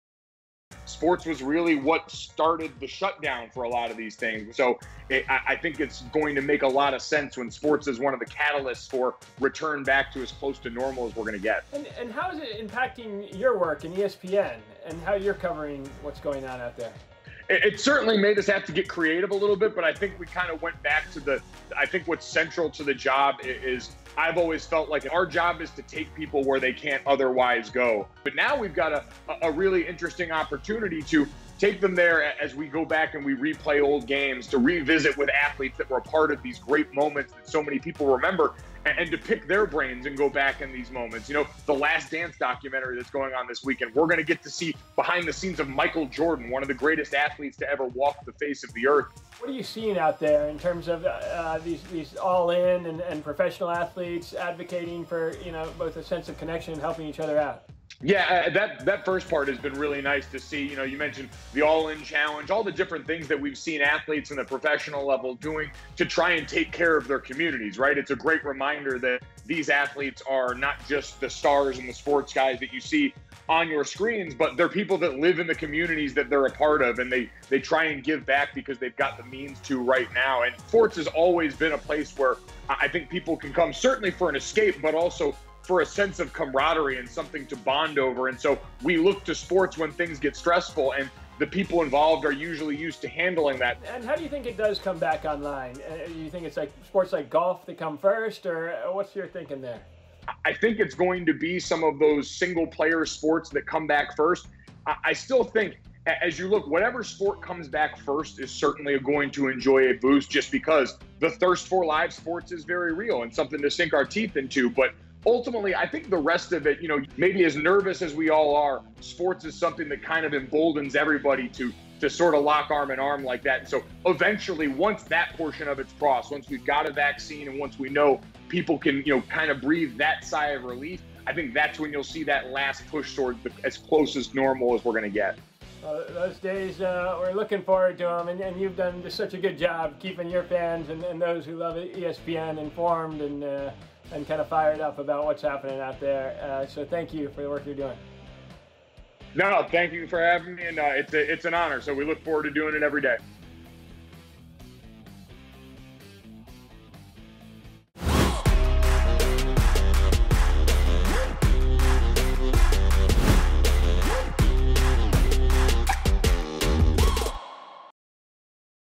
Checking in with ESPN's Mike Golic Jr. | More in Common
'I think It's going to make a lot of sense when sports is going to be the catalyst for a return back to as close to normal as we're going to get.' We check in with Mike Golic Jr. from ESPN about the future of sports and how ESPN is adapting to face the challenges in the age of COVID-19.